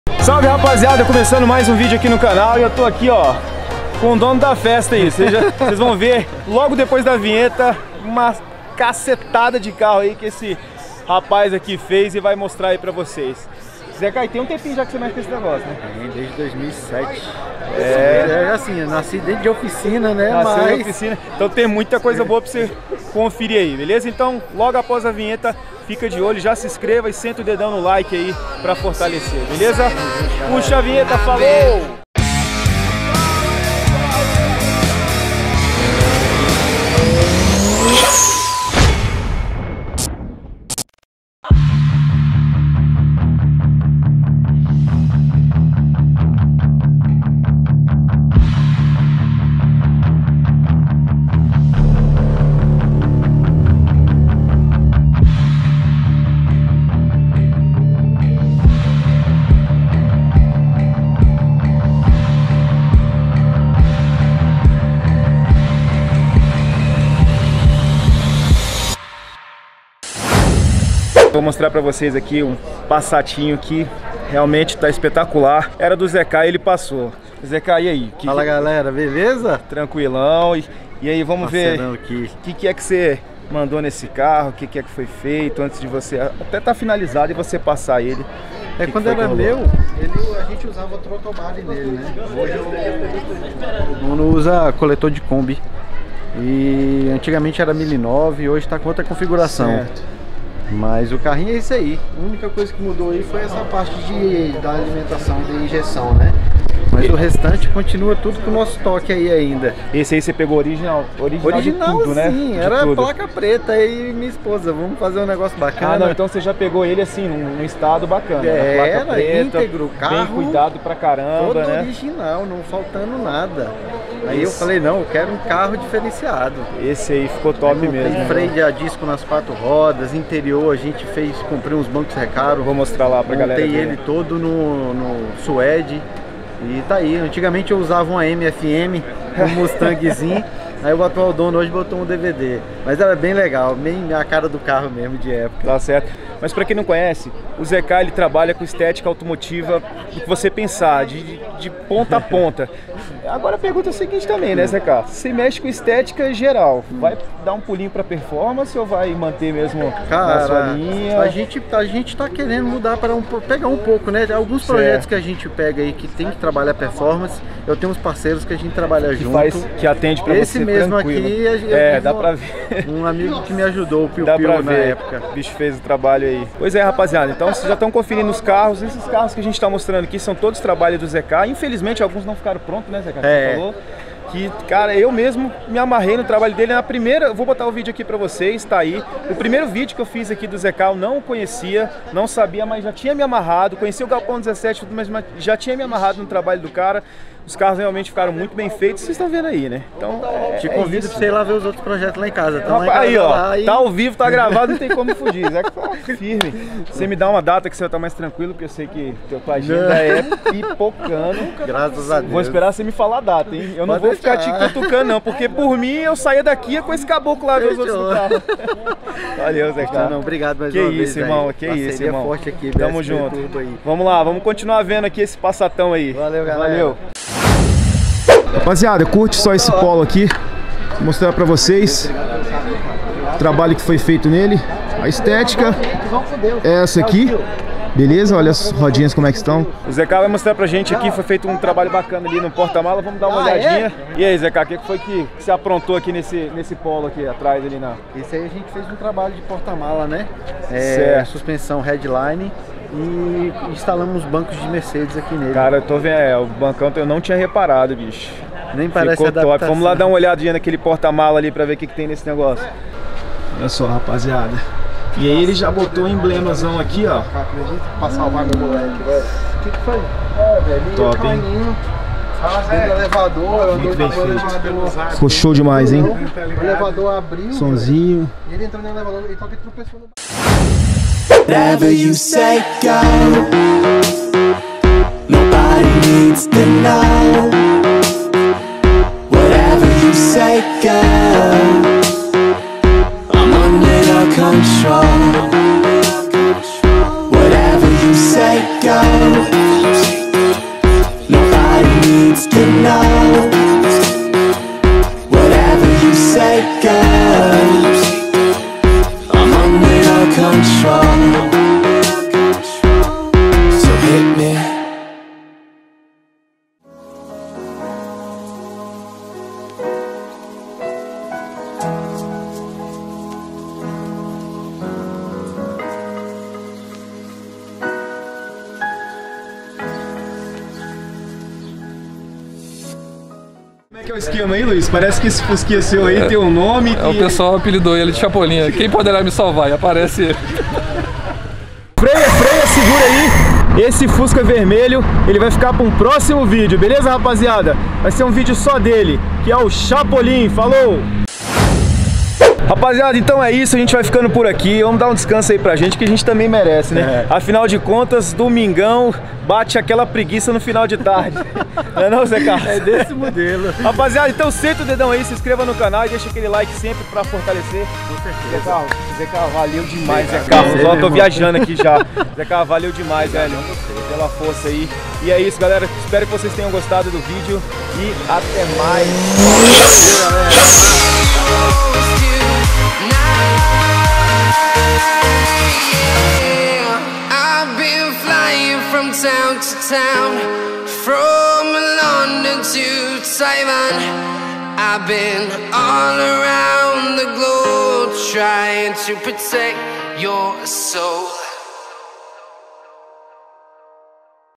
ESTÉTICA AUTOMOTIVA EXPOSIÇÃO ZECAR GALPÃO 17
Salve rapazeada, evento que aconteceu ontem dia 14 domingão.
Exposição do nosso parceiro Zecar, que trabalha com estética automotiva, e colocou os carros que ele montou para nossa apreciação! Espero que gostem...